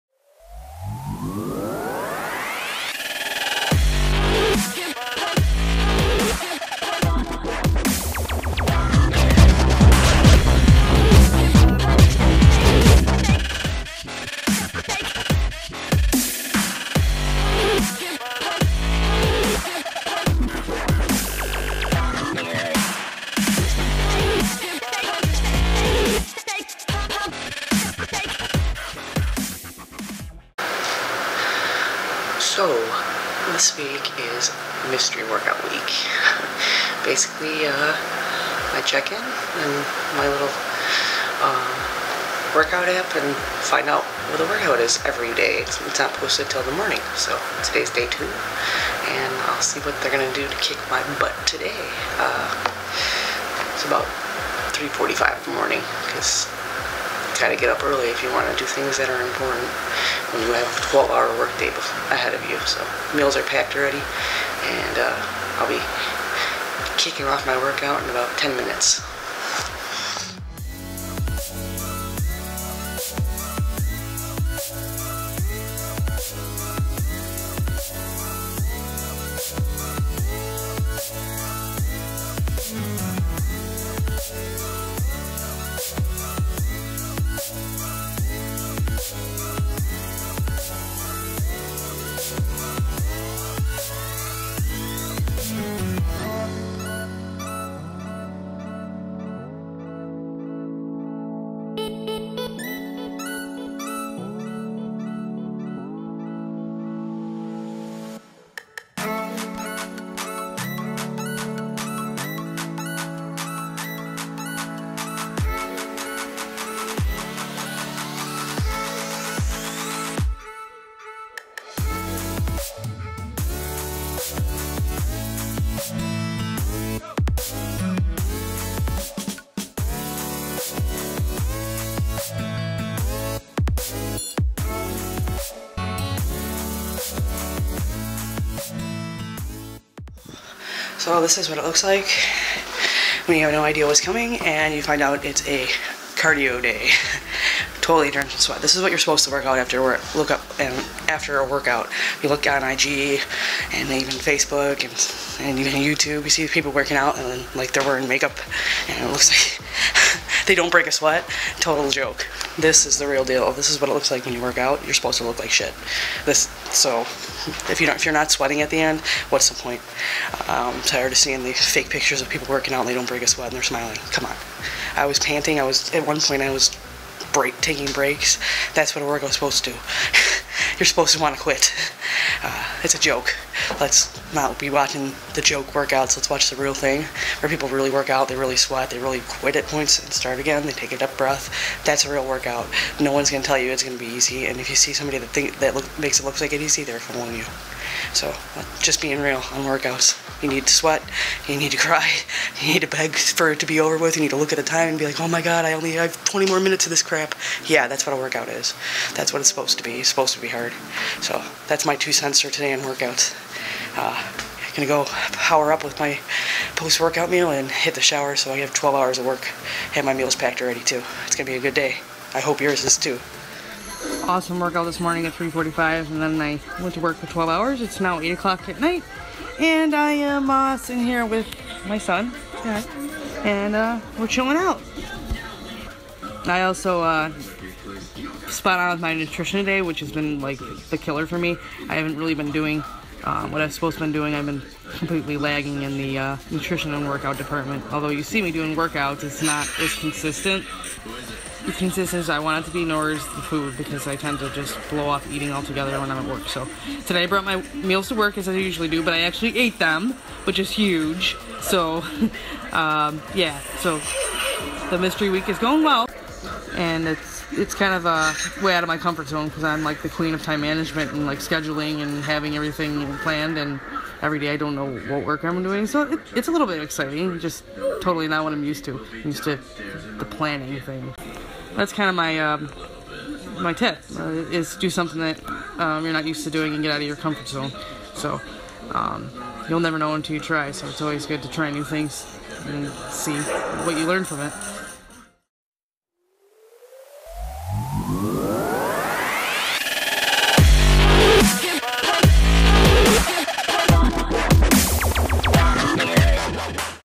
What Real Sweat Looks Like
In my vlog today I show you what real sweat looks like after a workout. It's not like you see in other fitness trainer videos and IG posts. Moreover, I talk about how you should look after a workout and getting out of my comfort zone with a mystery workout week.